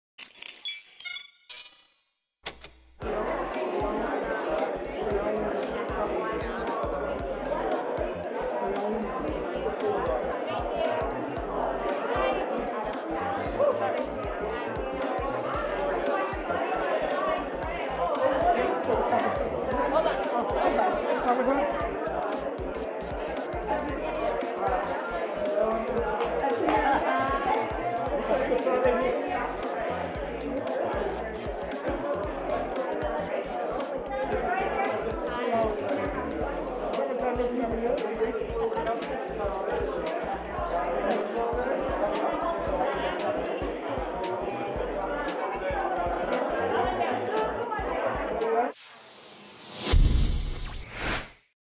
Riawna Capri 2019 'Hollywood Beauty Awards' Red Carpet
Broll footage: Hairstylist Riawna Capri on the red carpet at the 5th annual Hollywood Beauty Awards held at Avalon Hollywood in Los Angeles, California USA on February 17, 2019